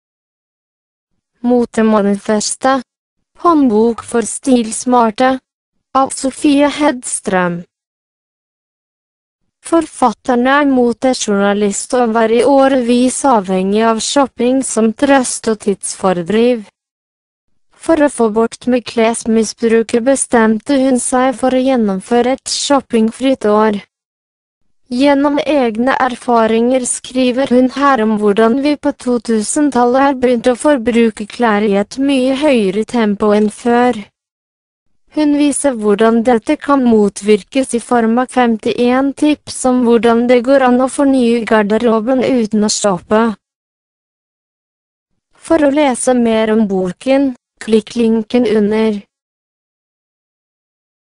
Motemanifestet - Sofia Hedström
Motemanifestet : håndbok for stilsmarte. Av Sofia Hedström. Forfatteren er motejournalist og var i årevis avhengig a...